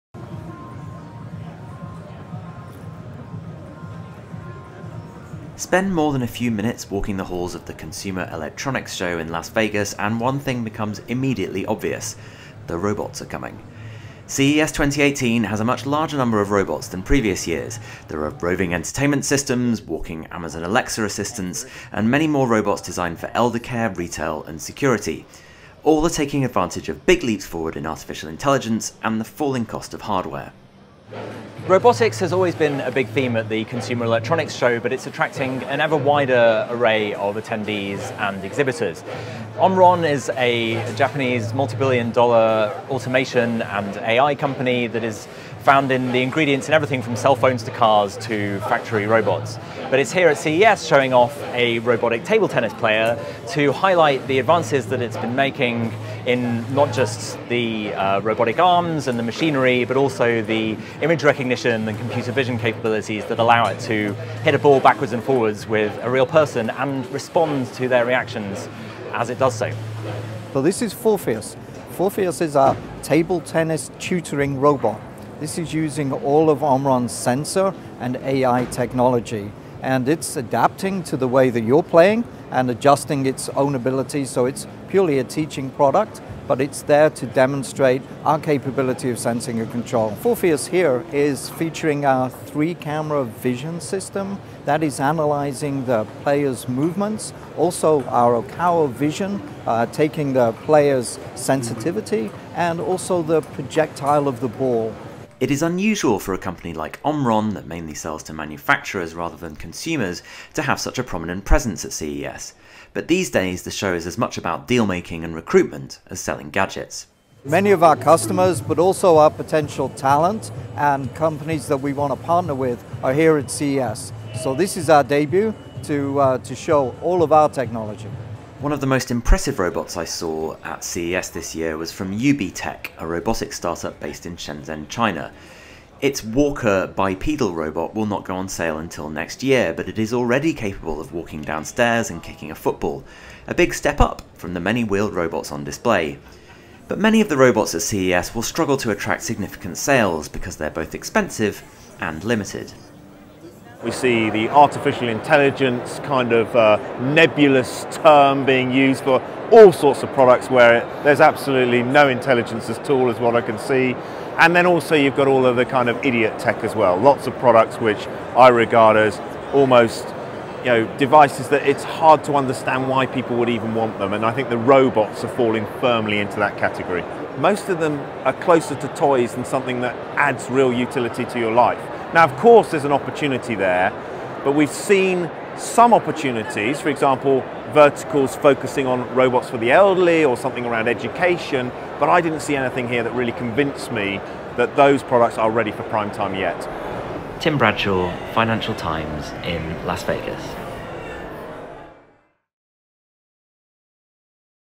Rise of the robots at US electronics show
The FT's Tim Bradshaw plays table tennis and football with the robots at the Consumer Electronics Show in Las Vegas to find out whether they are learning to play better with humans